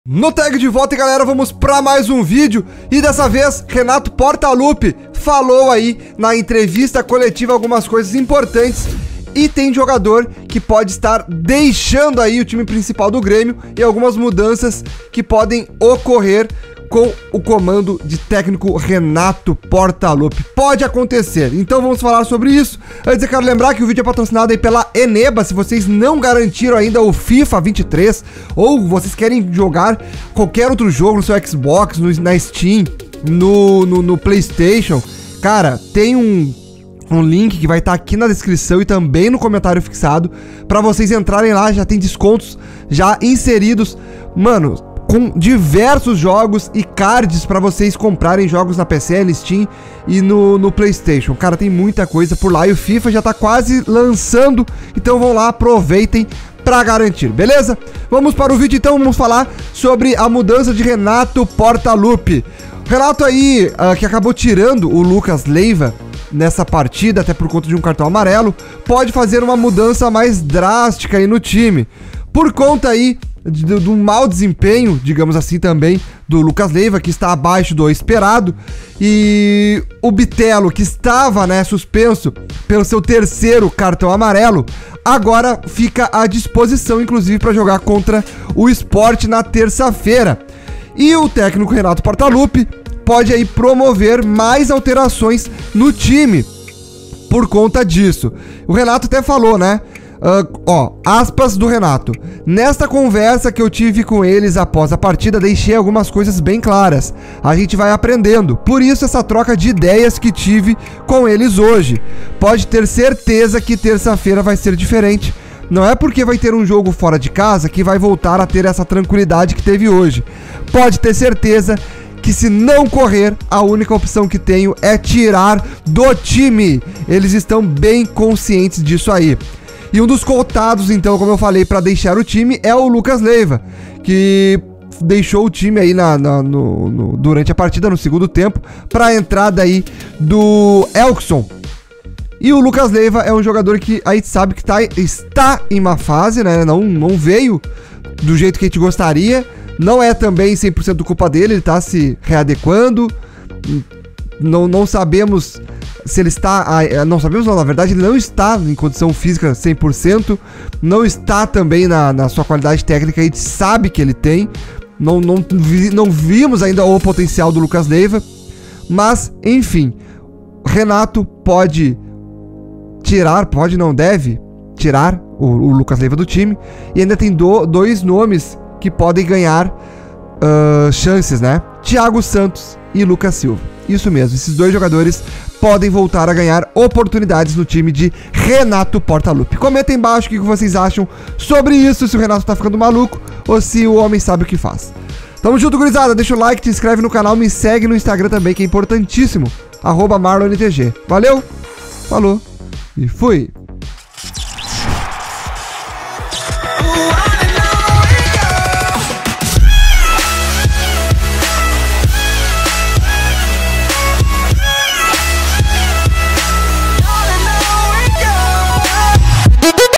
GRANDE MUDANÇA PODE OCORRER NO TIME DO GRÊMIO DE RENATO PORTALUPPI!
Renato pode fazer grande mudança no time do Grêmio!